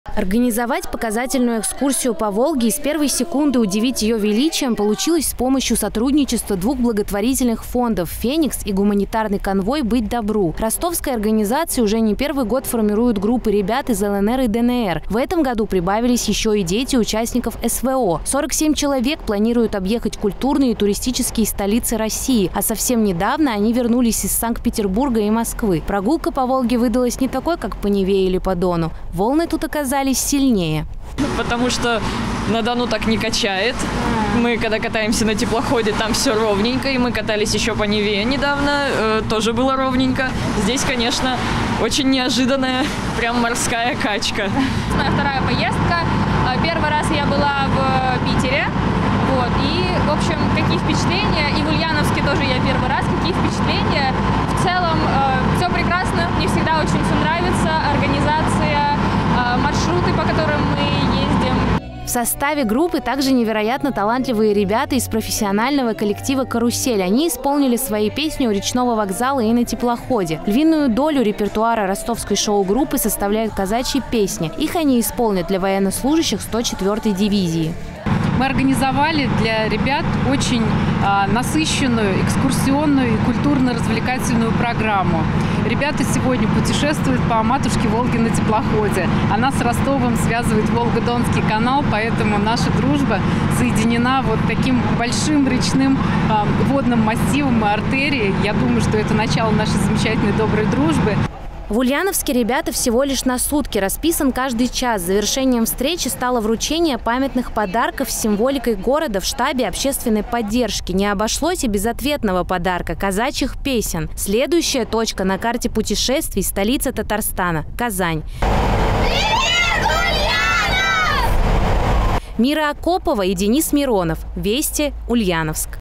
Дети из Луганска прокатились по Волге на теплоходе.
Ростовский фонд совместно с Ульяновской благотворительной организацией привезли в Ульяновск школьников из Луганской республики.